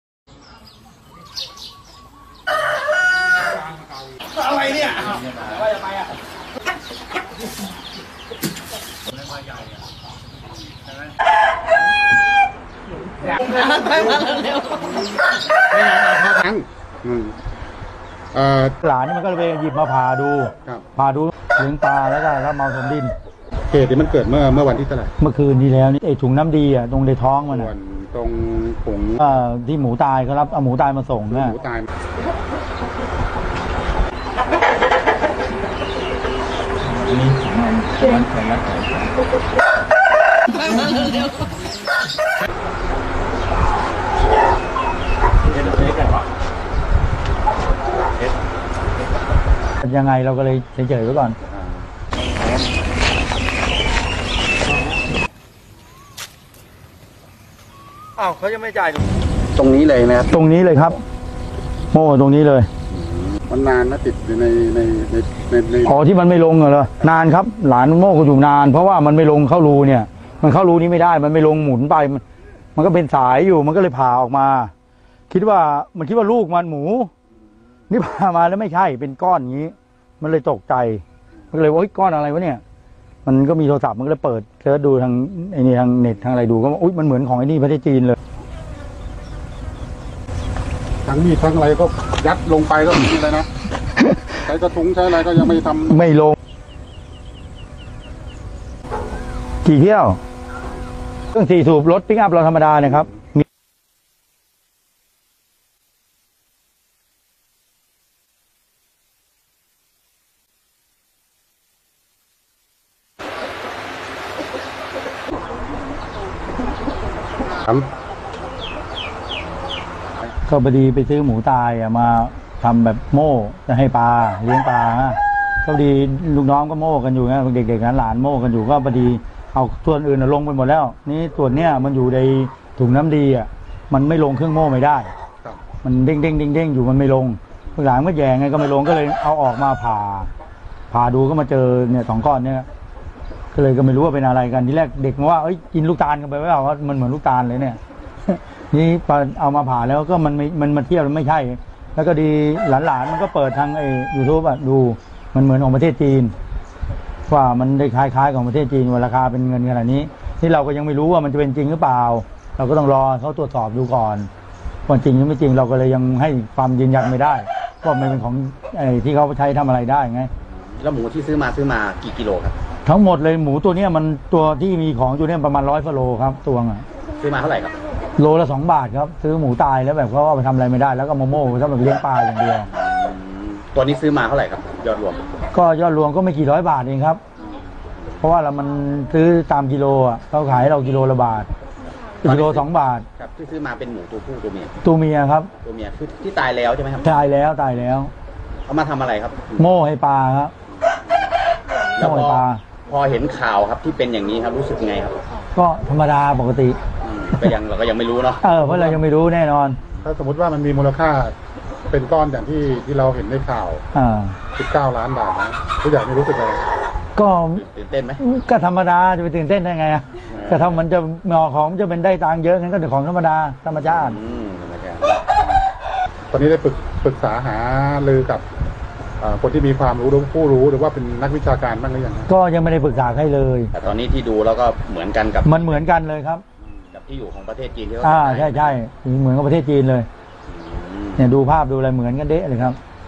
อือฮายิ่งกว่าถูกรางวัลที่ 1 ผญบ.พนัสนิคม เจอ “อ้วกหมู” คล้ายใยไหมสีทองเหมือนที่พบในจีน
อือฮายิ่งกว่าถูกรางวัลที่ 1 ผญบ.อ.พนัสนิคม จ.ชลบุรี เจอ “อ้วกหมู” ซึ่งเป็นเศษขนหมูจับตัวเป็นก้อนลักษณะคล้ายใยไหมสีทองในถุงน้ำดีหมูตายที่นำมาบดเลี้ยงปลา เผยเป็นแบบเดียวกับที่เป็นข่าวดังในจีน รอพิสูจน์ว่าใช่หรือไม่